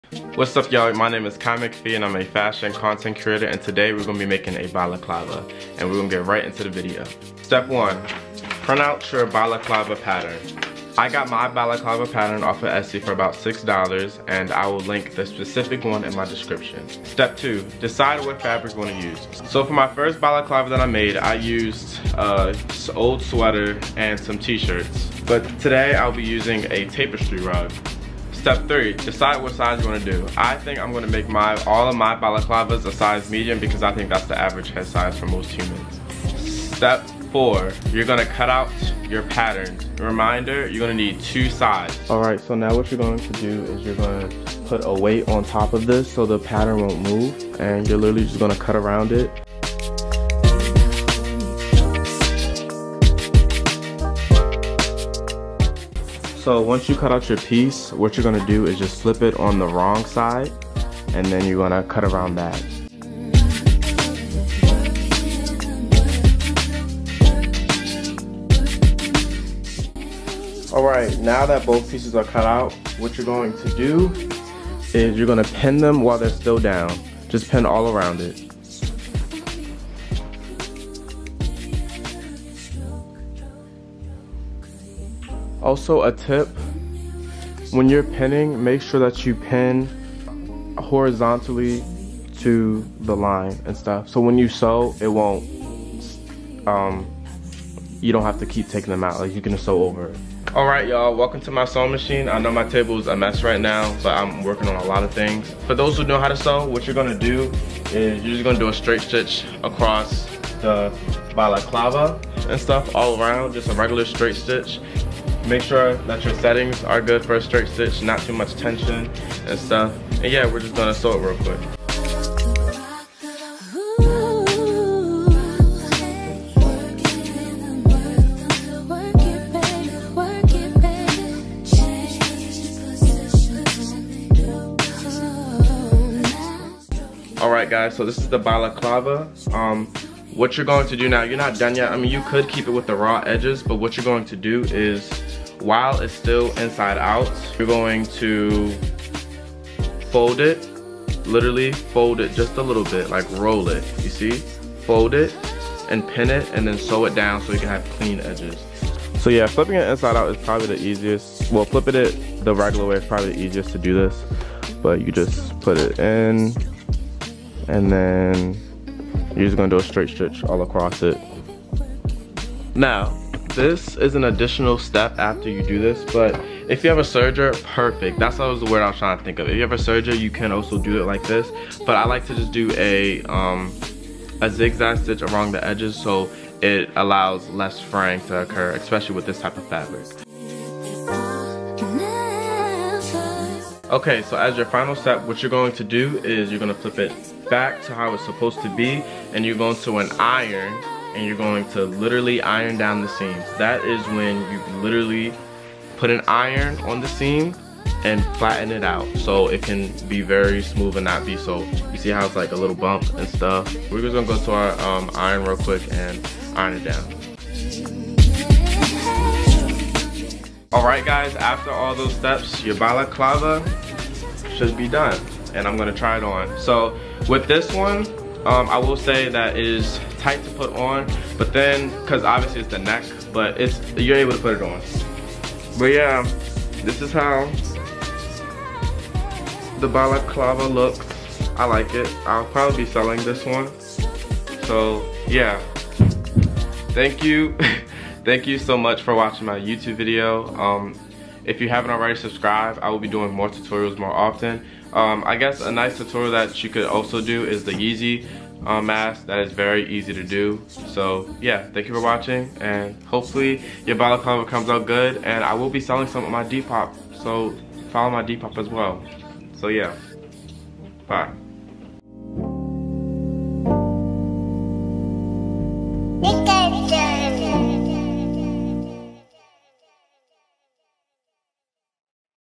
Balaclava Sewing Tutorial (EASY)
Hopefully this is super easy for you! I used a tapestry rug to make this one but you can honestly use any material as well; get creative!

Balaclava Pattern

Timestamps:
0:00 Intro
0:07 Step 1
0:17 Step 2
0:27 Step 3
0:36 Step 4
0:42 Step 5
1:15 Step 6
1:48 Step 7
2:25 Step 8
2:54 Step 9
3:13 Step 10
3:40 Outro

Socials:
Depop: vibinwkai

Also Happy Birthday Aaliyah :)